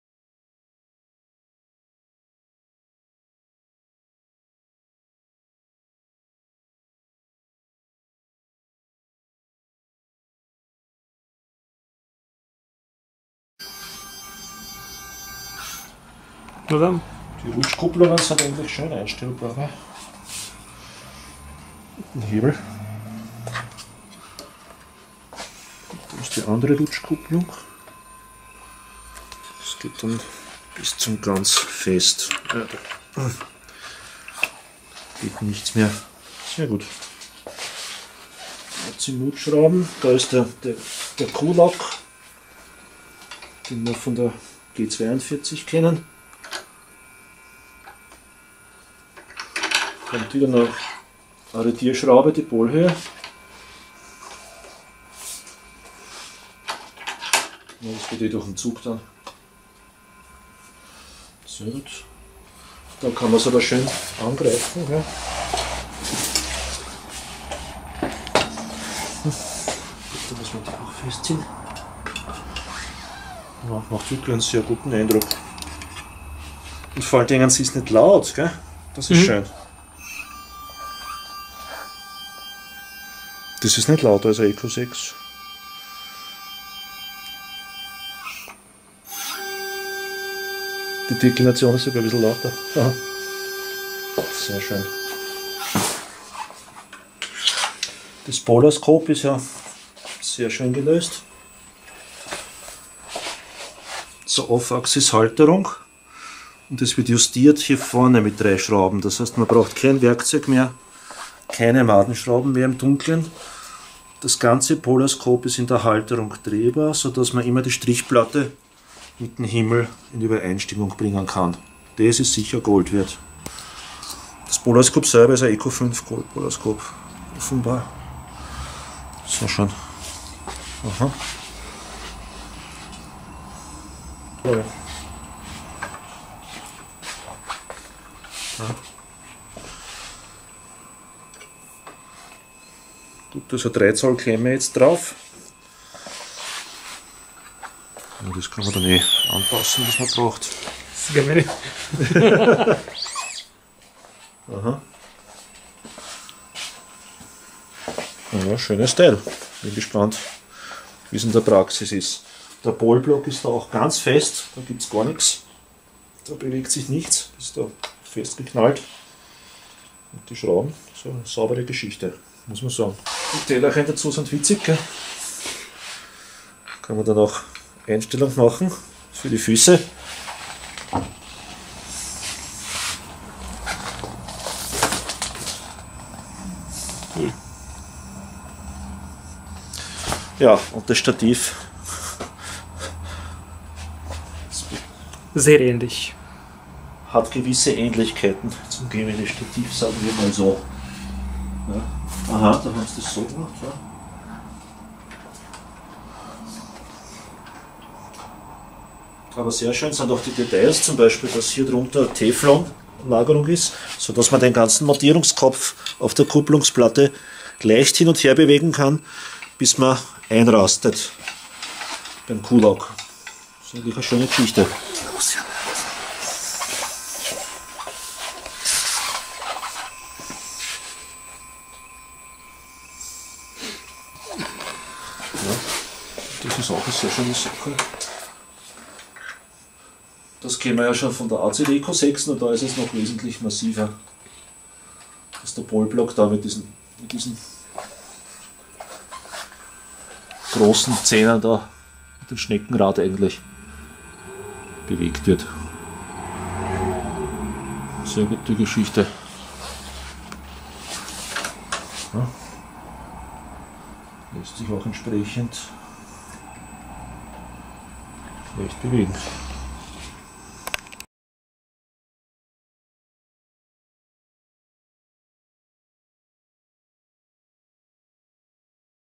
EQ8 - der erste Eindruck
SkyWatcher EQ8 equatorial mount - first impressions - by Teleskop-Austria. Please use cc button for english - guiding and PE graphs here: